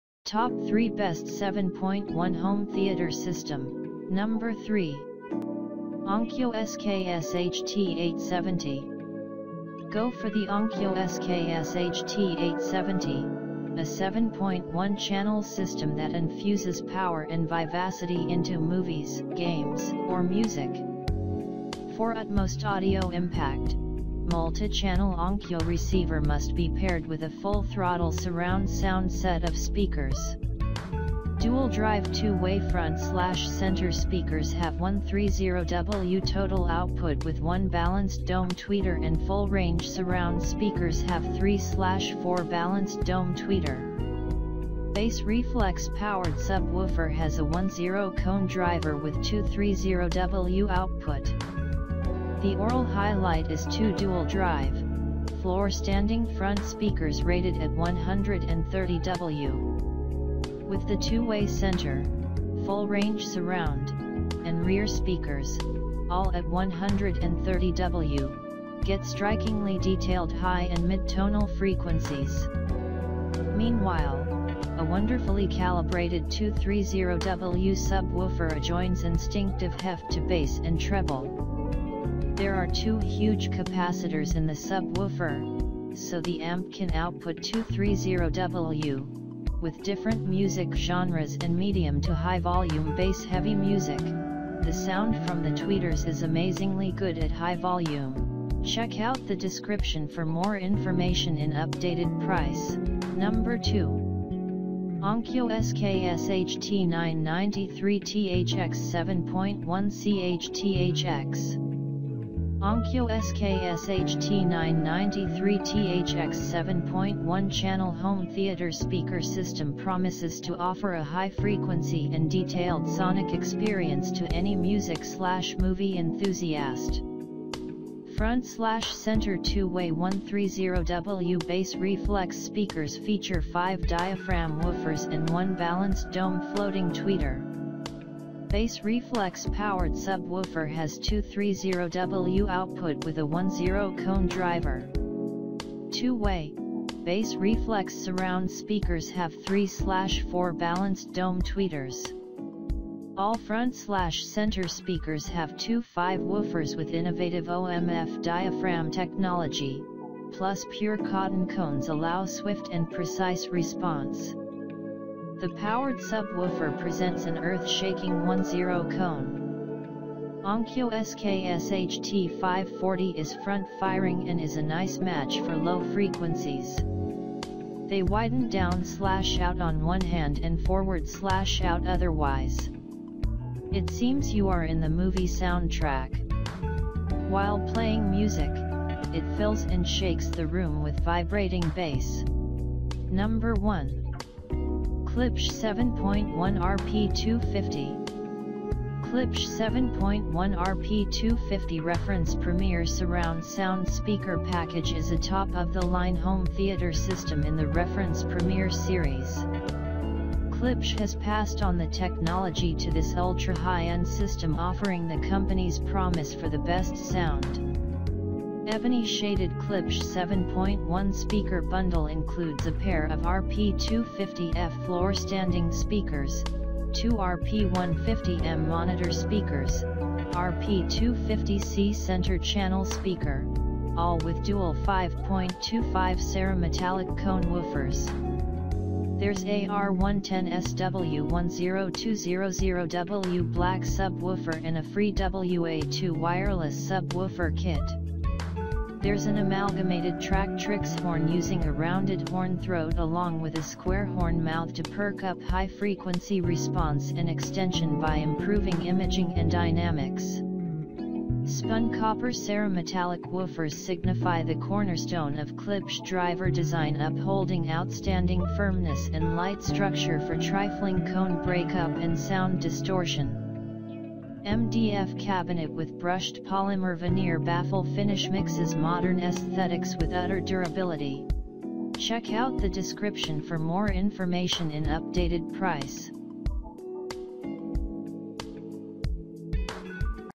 Top 3 Best 7.1 Home Theater System 2023
► Top 3 Best 7.1 Home Theater System 2023!These Links For Updated Price Below.
❥ 1. Klipsch 7.1 RP-250

❥ 2. Onkyo SKS-HT993THX 7.1 Ch. THX

❥ 3. Onkyo SKS-HT870